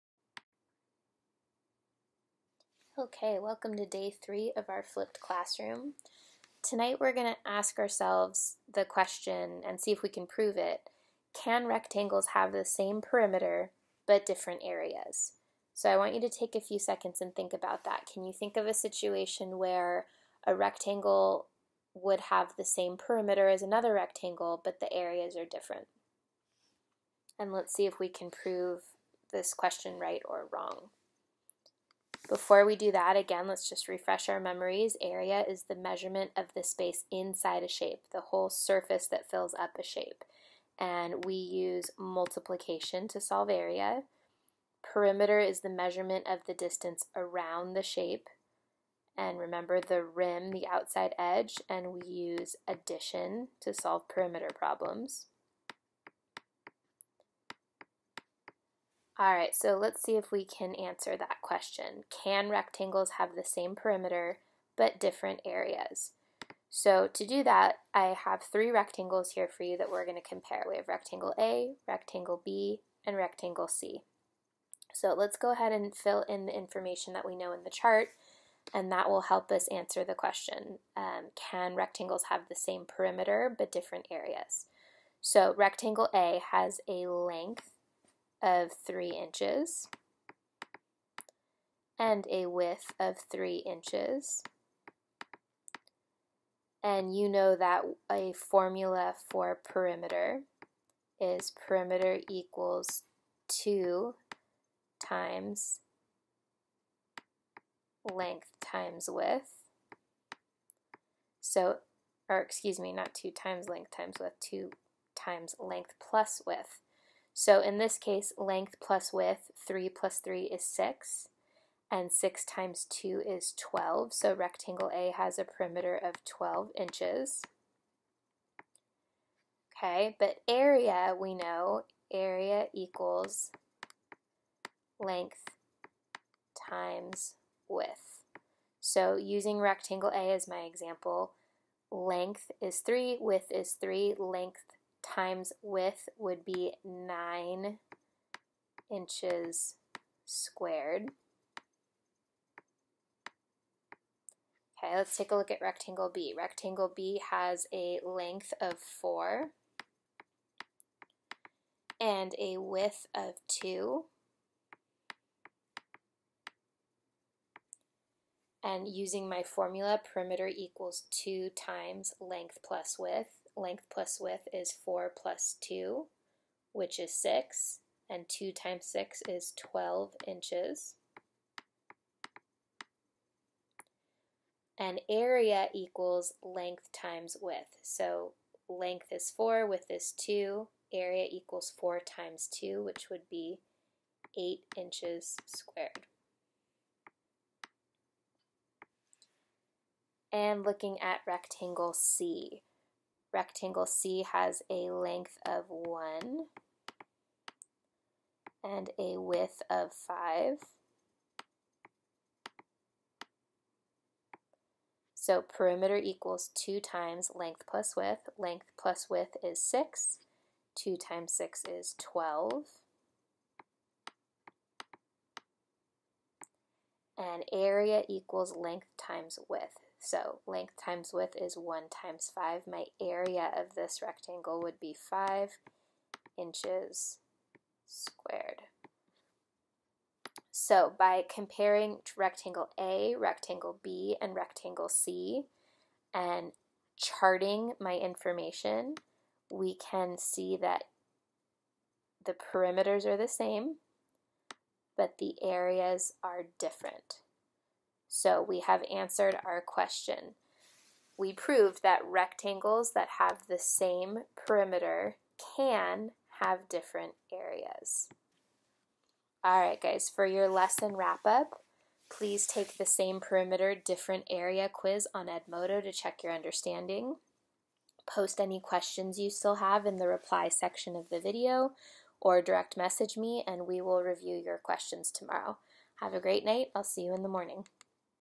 Same Perimeter, Different Area
Can rectangles with the same perimeter have different areas?